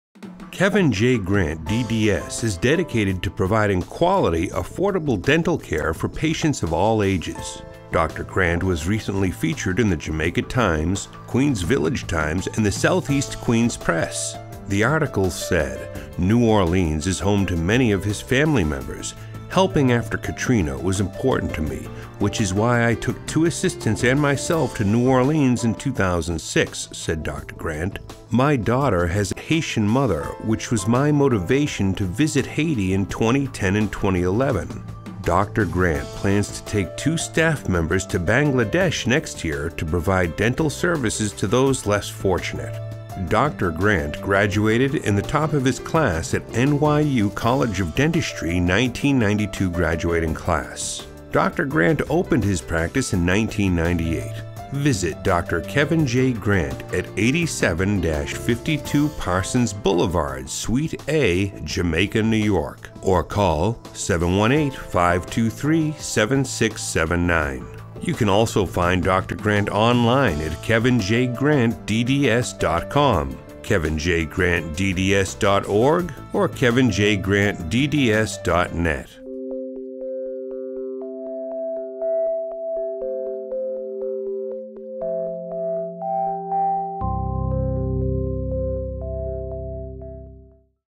How to Create Promotional Videos for Dentists
How to Create Promotional Video: This is an actual video we
produced for a dentist client to help them
preserve and spread their great news with the power of video. We
also provide a professional guide teaching our clients how to
promote YouTube videos.Promotional videos give businesses the
opportunity to celebrate on their YouTube channels. Once live, they
can promote YouTube videos by sharing on their blog and social
media.